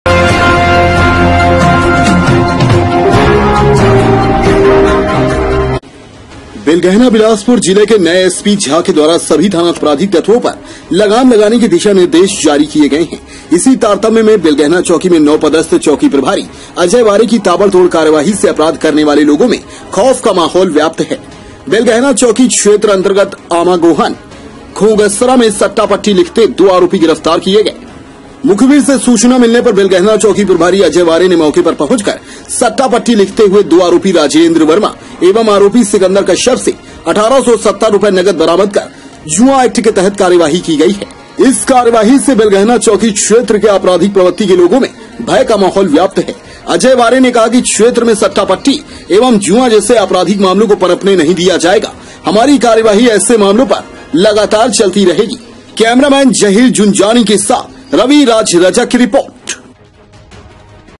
बेलगहना: शहर से गांव तक पहुंचा सट्टा पट्टी का कारोबार सट्टा पट्टी लिखते दो आरोपी गिरफ्तार
शहर से गांव तक पहुंचा सट्टा पट्टी का कारोबार सट्टा पट्टी लिखते दो आरोपी गिरफ्तार